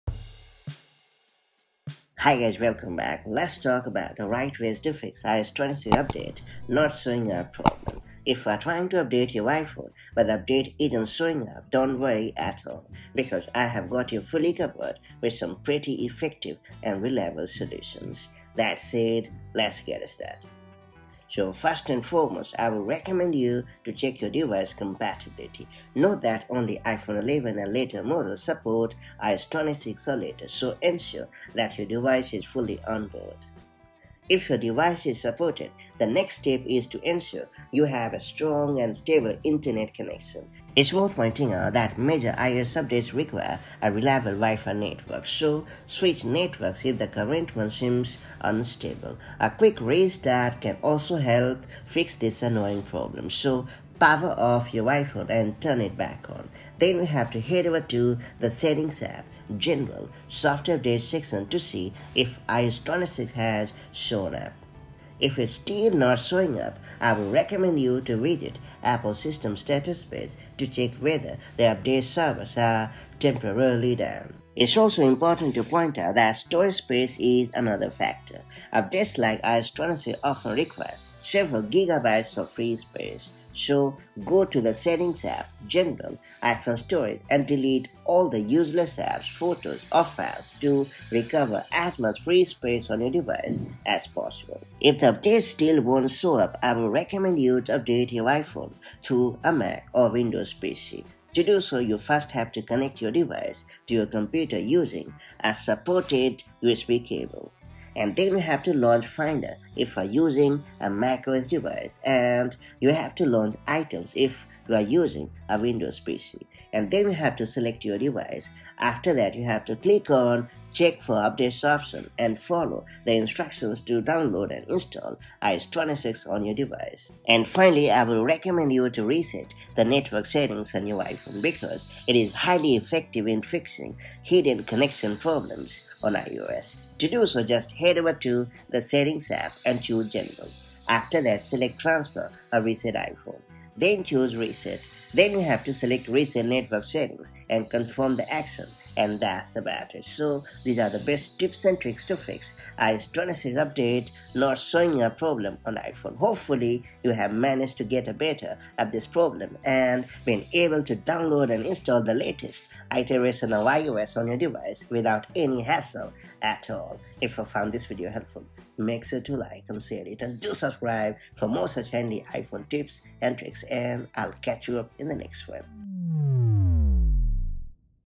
How to Fix iOS 26 Update Not Showing Up on iPhone & iPad
Is the iOS 26 update not showing up on your iPhone or iPad? Don’t worry—this video walks you through the best ways to fix it quickly. Learn how to refresh your device, check compatibility, reset network settings, and update manually so you can install iOS 26 without delays. Whether your iPhone or iPad doesn’t show the update or gets stuck, these solutions will help you fix the issue in minutes.